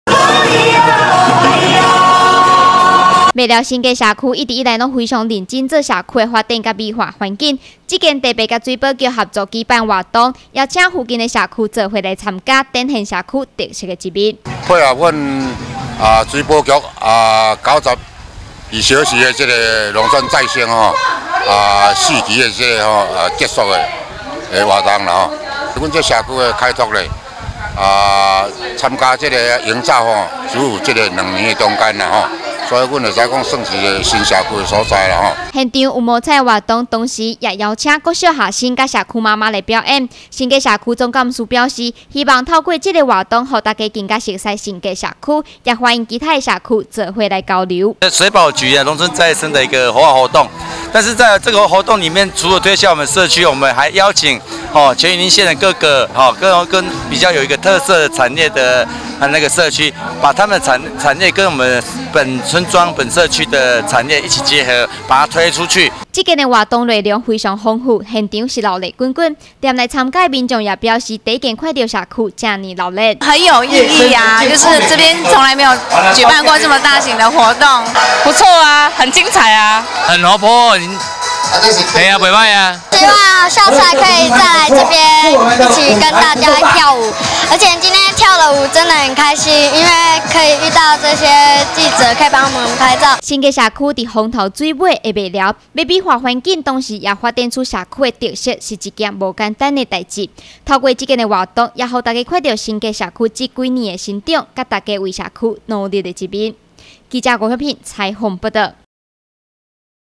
雲林新聞網 麥寮新吉社區活動
麥寮新吉社區配合水土保持局活動，特別舉辦一系列的表演摸彩活動，除了邀請鄰近社區設攤位，將他們社區特別的特色展現出來以外，還邀請學校小朋友跟社區媽媽來表演，現場是熱鬧滾滾，前來參觀的民眾紛紛表示，表演非常精彩，新吉社區未曾這麼熱鬧過。